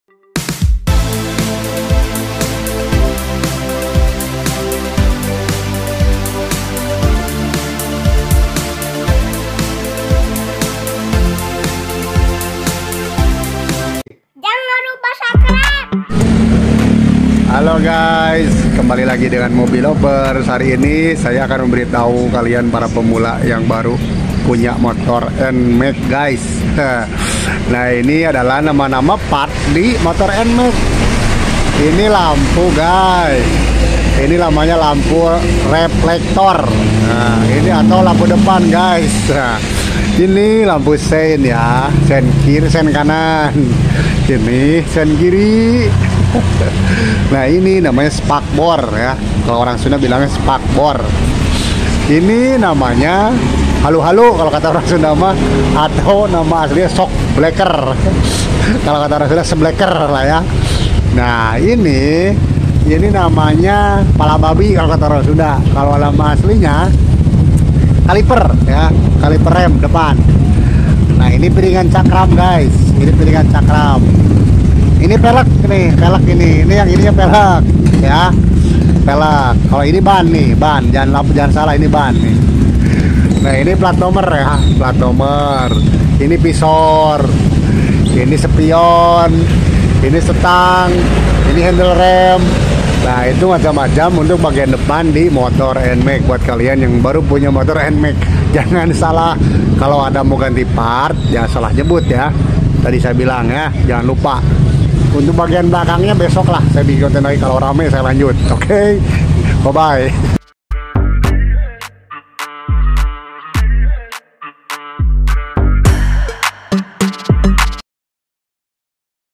Latest Funny Video "NAMA" N-MAX MOTORCYCLE PARTS Beginners Must Know ‼️
Macam - Macam Nama Part Di Motor N-Max Pemula Wajib Tahu ‼️
Info terbaru tentang Part Motor N-max
Cocok untuk pemula
semoga Bermanfaat